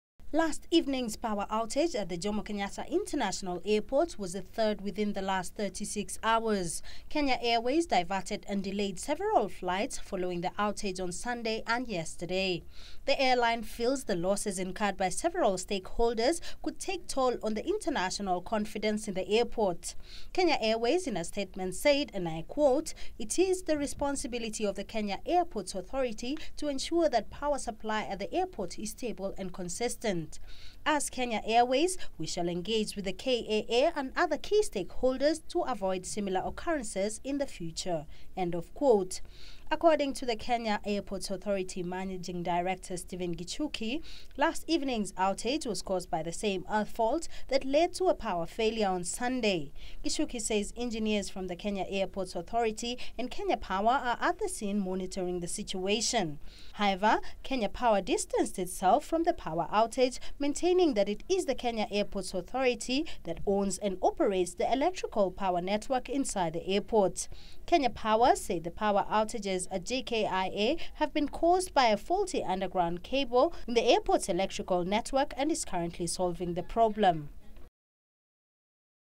JKIA hit by third power outage in 2 days
The Jomo Kenyatta International Airport (KJIA) was faced with the third power outage in 36 hours at around 7 p.m. Monday evening, causing the national carrier, Kenya Airways to redirect its flights to other airports.
KQ confirmed that flights KQ 121 from Rome and KQ 763 from Johannesburg had been diverted to Mombasa International Airport and KQ 500 from Libreville diverted to Entebbe International Airport.
Flight KQ 613 from Mombasa to Nairobi was held on the runway at the Mombasa International Airport.
While strongly decrying the flight disruptions and inconvenience to passengers occasioned by the recent power outages at JKIA, Kenya Airways MD and Group CEO Mr. Naikuni said that Kenya Airways will continue to reroute all passengers who missed connections to their final destinations at no extra cost.
"In the event that there were no immediate connections available, Kenya Airways would provide the passengers with hotel and accommodation and book them on the first available flights to their final destinations," he added.
"It is the responsibility of Kenya Airports Authority to ensure that power supply at the airport is stable and consistent. As Kenya Airways, we shall engage with KAA and other key stakeholders to avoid similar occurrences in future," said Naikuni.
The KQ CEO strongly requested all responsible people within Kenya Airports Authority and Kenya Power to make sure that Kenya does not loose international confidence as an aviation hub by resolving whatever the problem is with the urgency it deserves.
 "We have lost a lot of money as KQ and this cannot be allowed to continue for long", he said.
Meanwhile, the Kenya Airports Authority has announced that services at JKIA that had been interrupted by a power outage at about 6.40 p.m. Monday have returned to normalcy after a 30 minutes power outage.
KAA managing director Stephen Gichuki confirmed that engineers from the Airports Authority and Kenya Power had identified that the same earth fault that led to a power outage on Sunday had resulted in the subsequent power outage.
He further said that engineers are at the scene monitoring the situation.uploaded by E.Nyandoro.